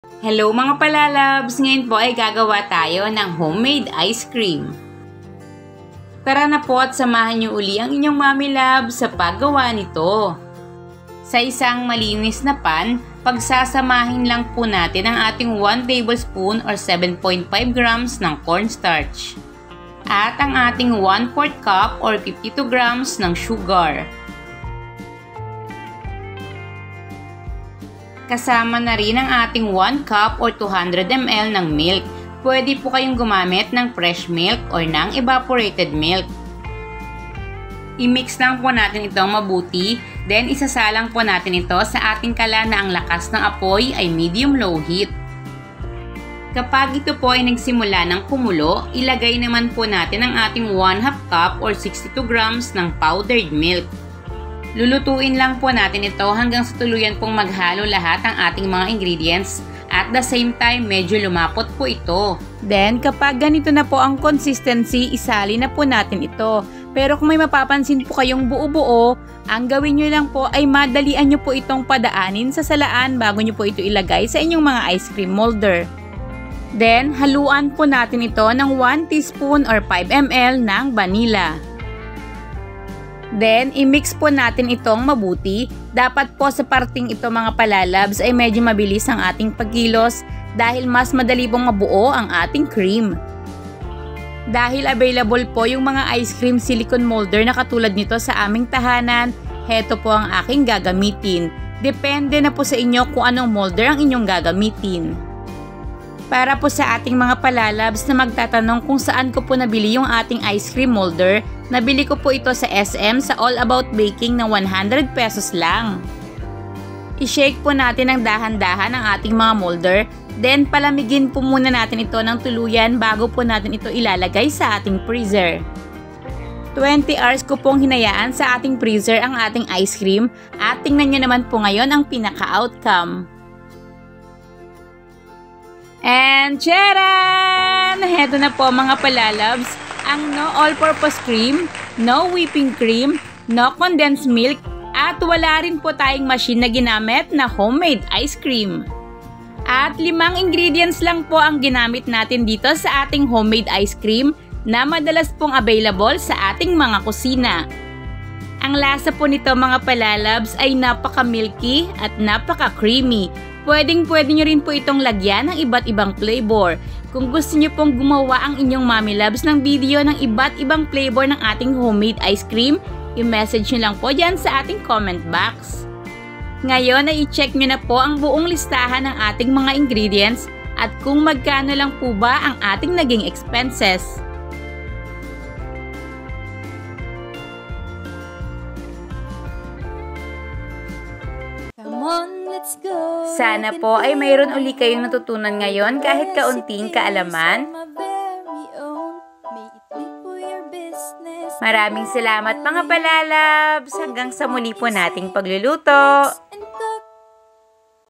Only 5 Ingredients Homemade Icecream | Mix N Cook
5 Ingredients Ice Cream | Mix N Cook

Hello! Today, we're going to make Ice Cream using 5-ingredients only. This dessert recipe won't be needing any cream, condensed milk, or even machine. Let's go!

Ingredients:

1 cup or 200 ml milk
1 tbsp. Or 7.5 g cornstarch
1 tsp. or 5 ml vanilla
1/4 cup or 52 g sugar
1/2 cup or 62 g powdered milk

For the complete costing and possible profit, refer to the end of the video

Mga Palalabs para po mas madali nyong mahanap ang inyong mga gustong recipe sa ating YouTube Channel nakalagay na po lahat sa playlist 👇👇👇